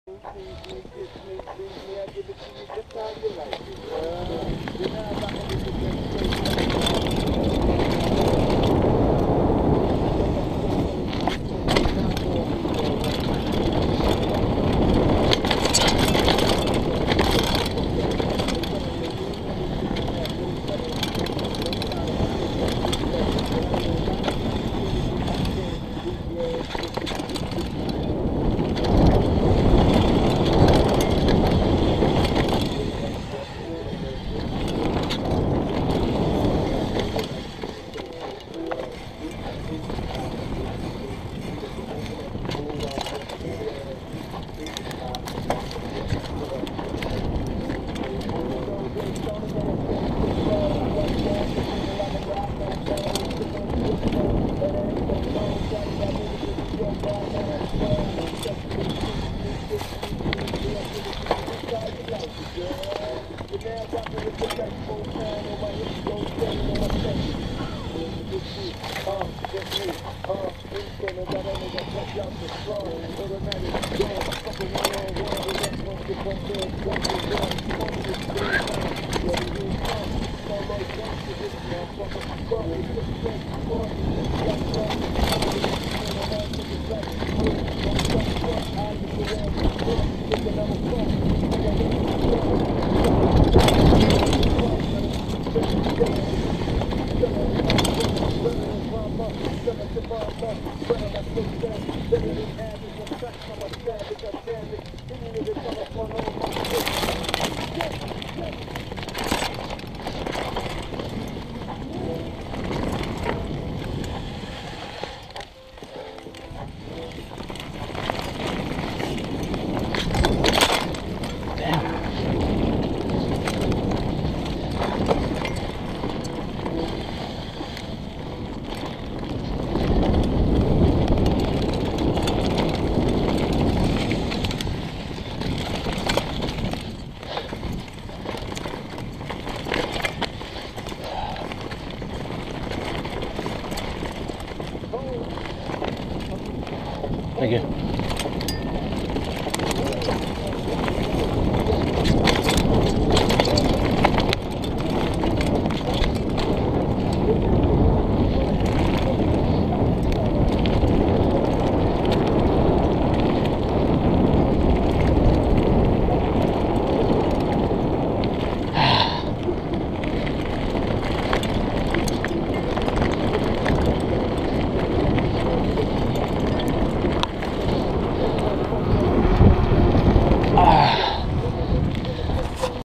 Block trail,corona, skyline
Down block trail@ skyline corona ca 01/07/17 first ride of the year, after some light rain still a bit muddy, fun trail